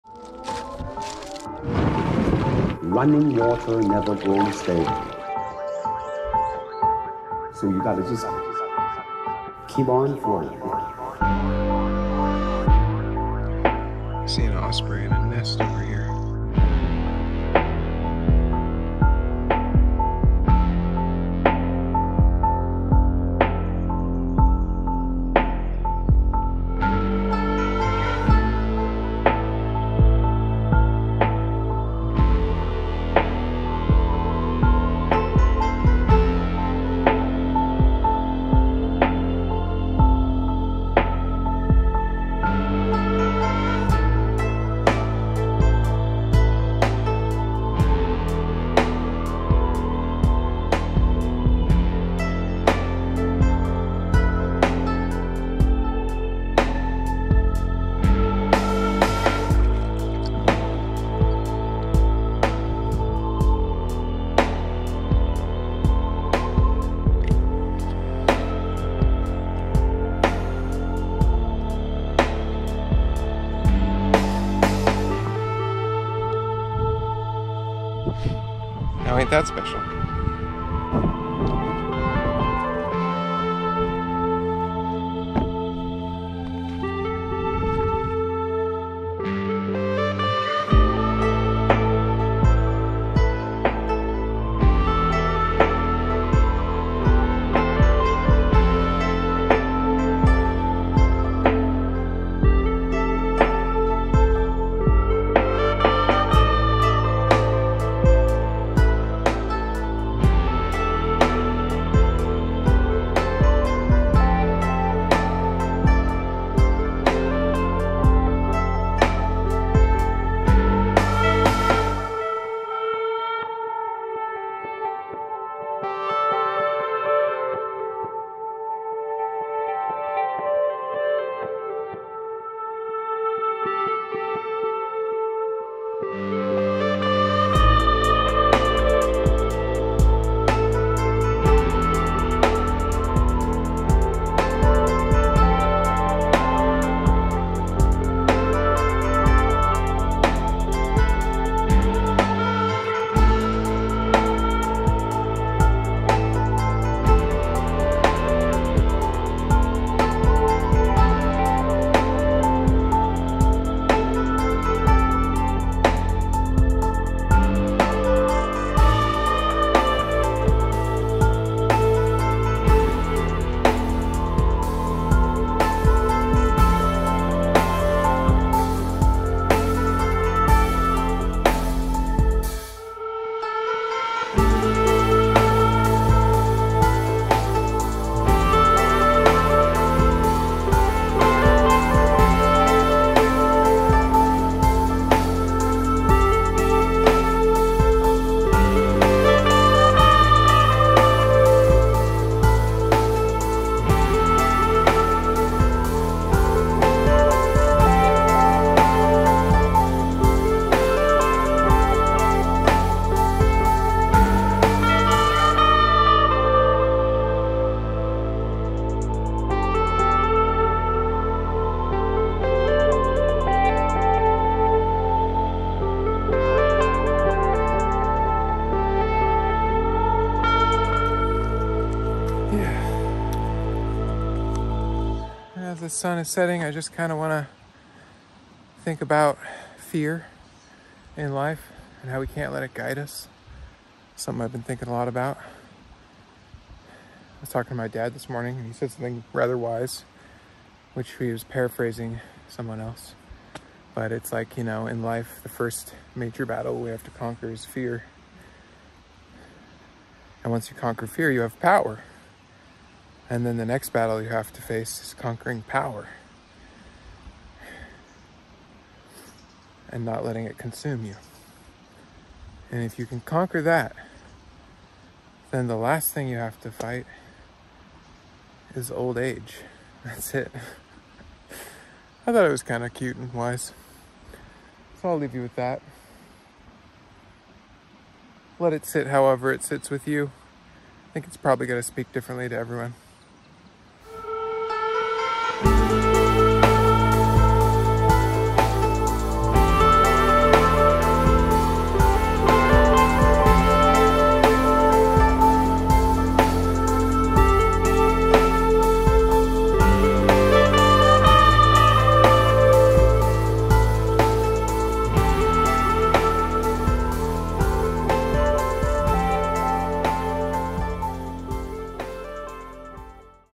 Practicing Martial Arts | Ospreys Catch Fish Nearby
Willamette River, Oregon
Filmed, Edited, and Soundtrack by me  @howlermonkeymusic

All music was performed, recorded, and produced by Howler Monkey at Polymorphic Productions (c)2023